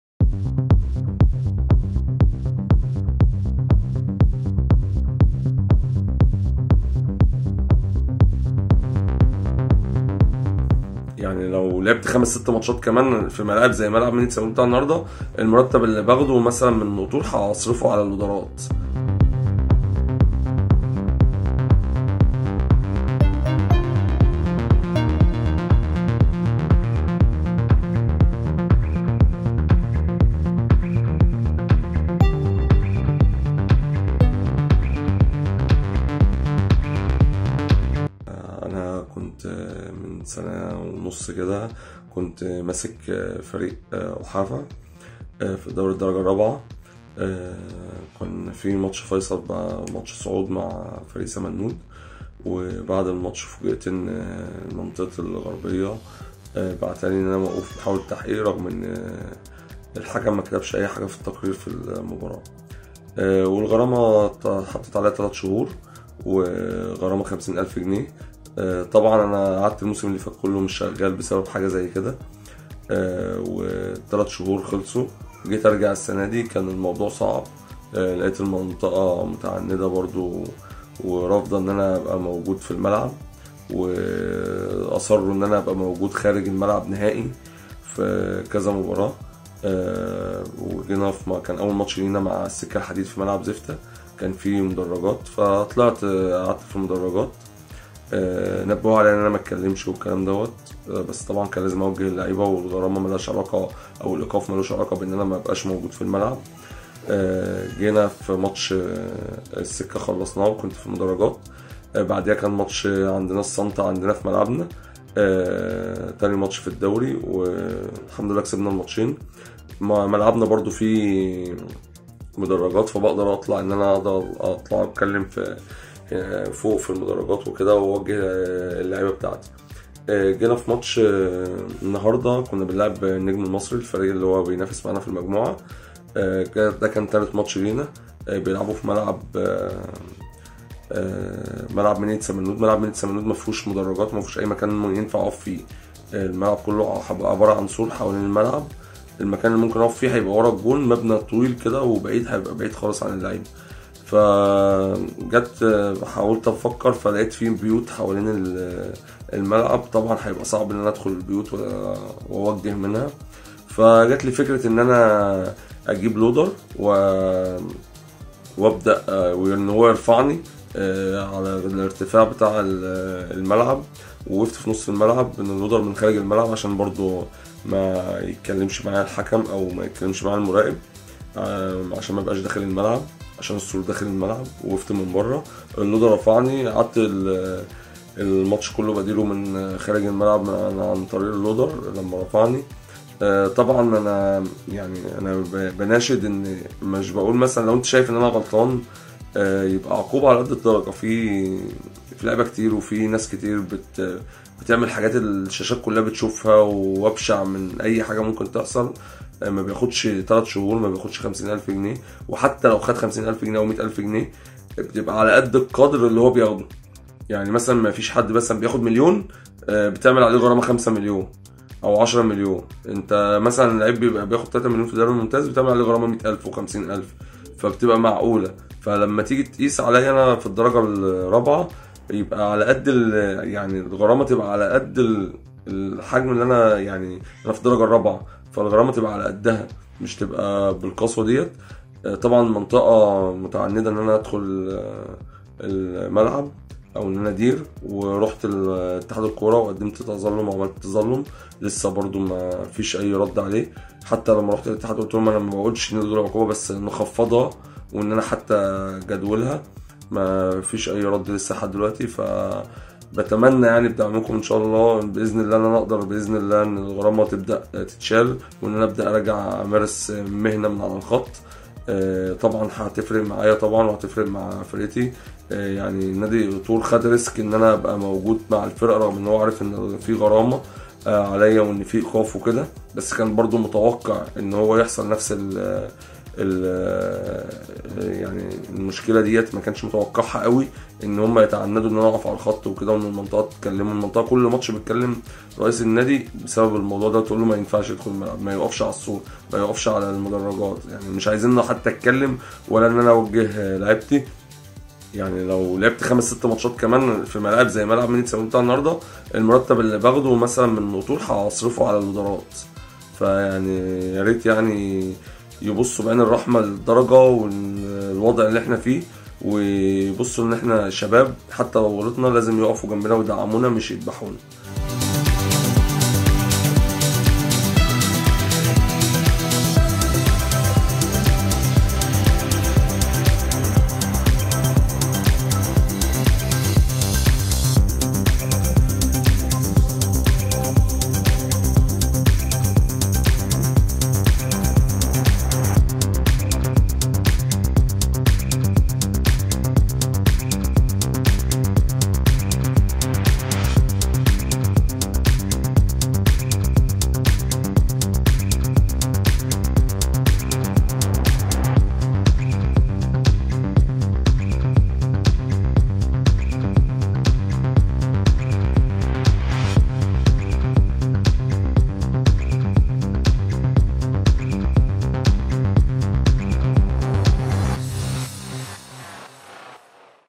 على طريقة سامبولي - مدرب مصري يقود مبارة لفريقه من على "لودر"
بسبب إيقافه❌

 محمد حسنين إينو مدرب مركز شباب قطور بدوري الدرجة الرابعة لفت الأنظار إليه.

على طريقة سامبولي، المدرب قاد مباراة فريقه من على "لودر"🏗

"لو لعبت 5 - 6 ماتشات في ملعب زي بتاع منية سمنود من غير مدرجات، المرتب اللي باخده من النادي هصرفه على اللودرات".🗣️